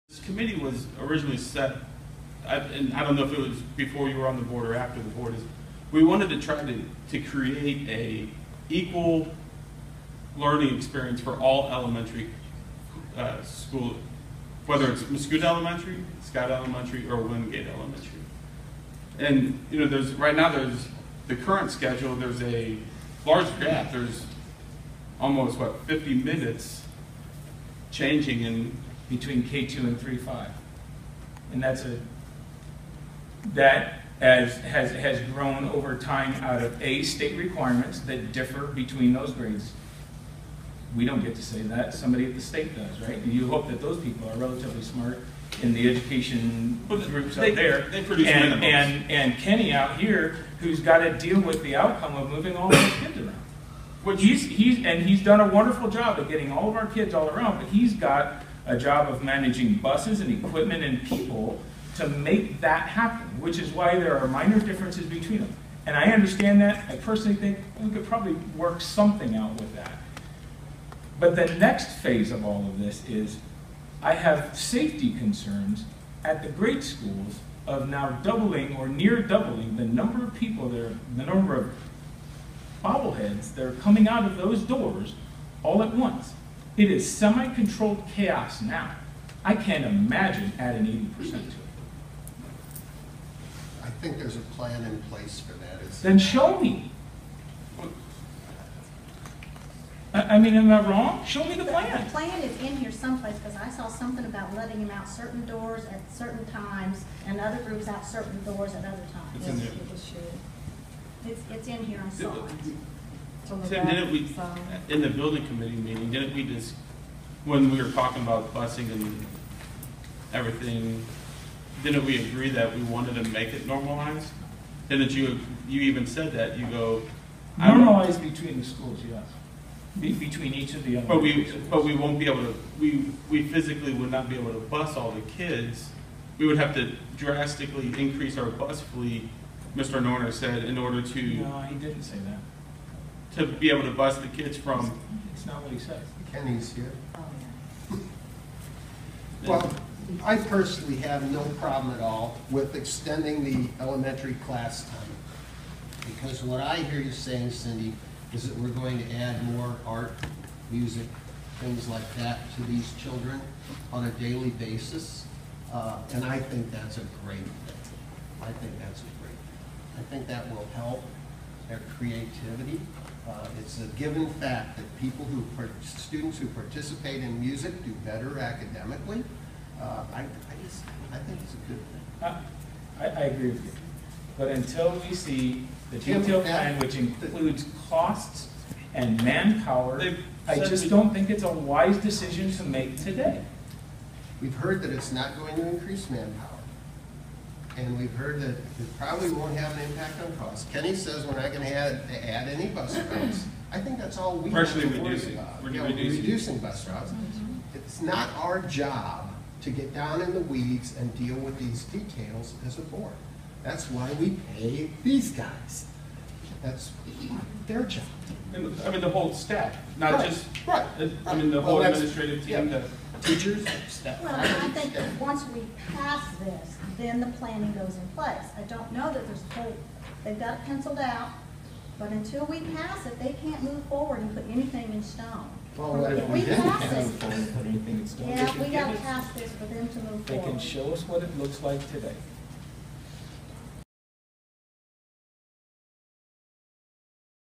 Changing School Start Times In District 19
The Mascoutah District 19 school board member Tim Petersen questioned the proposed change at the meeting on Tuesday night. Board president Gregory Guenther and members Matthew Stukenberg and Susan Albrecht were among those defending the proposal, which passed with only Petersen dissenting.